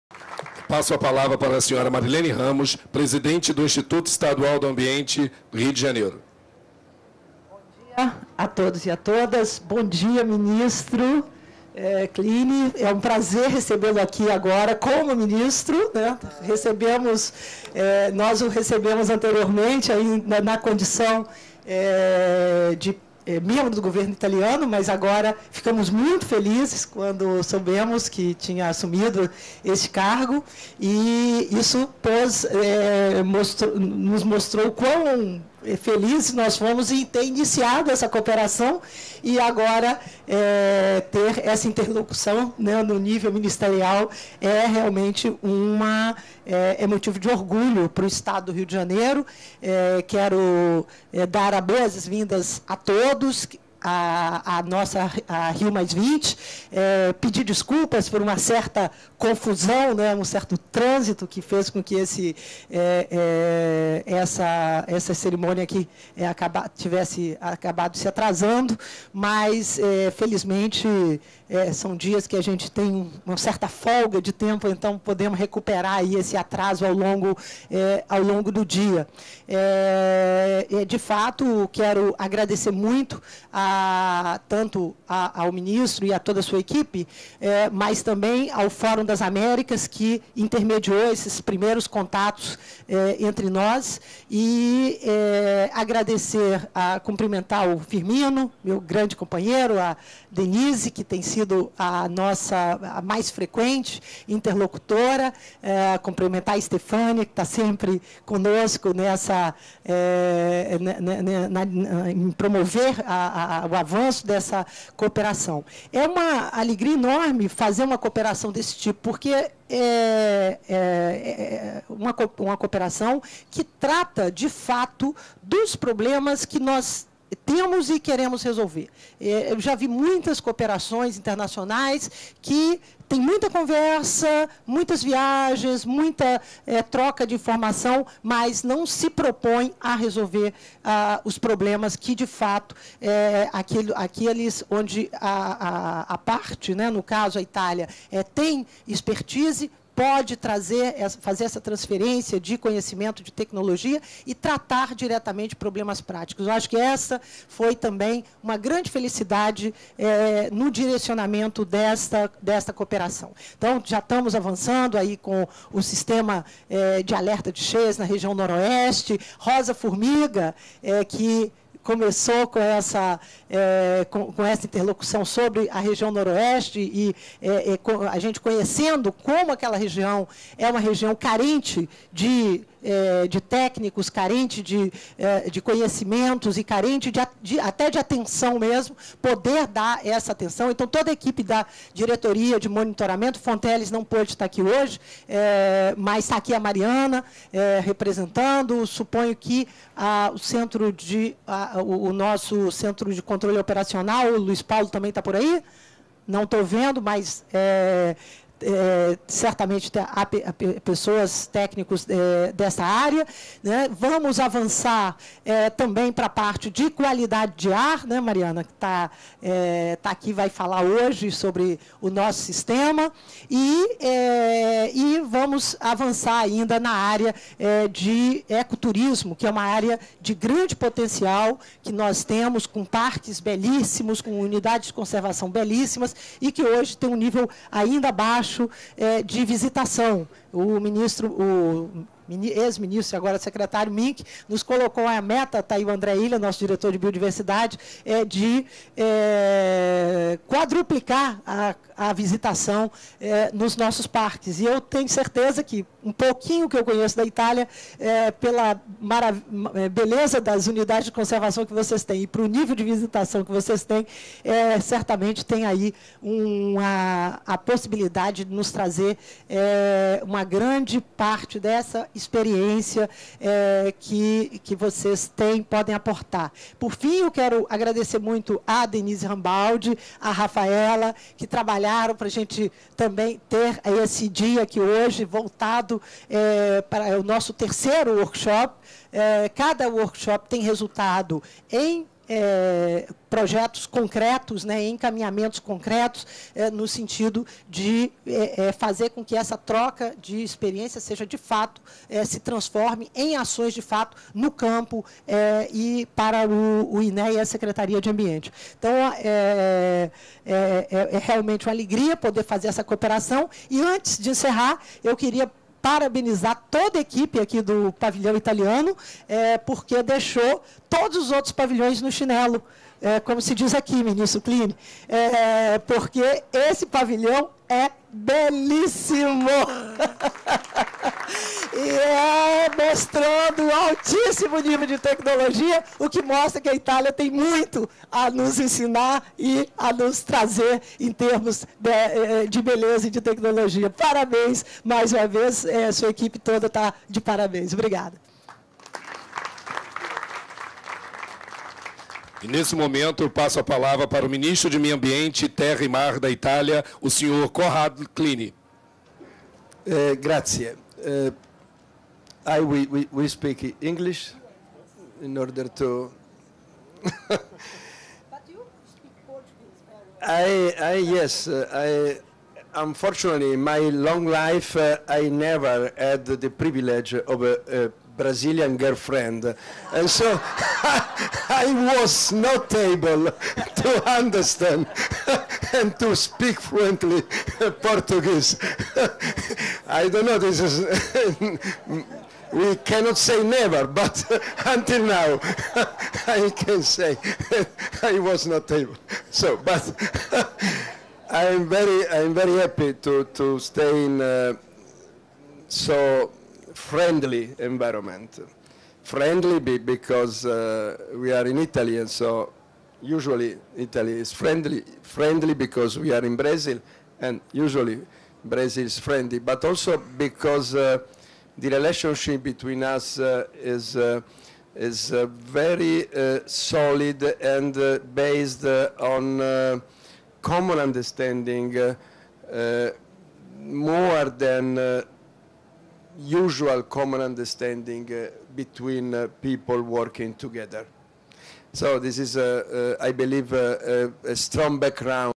19.06.2012 - INEA: Italy - Rio de Janeiro Enviromental Cooperation Programme - Marilene Ramos (3)
Marilene Ramos, Luiz Firmino, Denise Rambaldi, INEA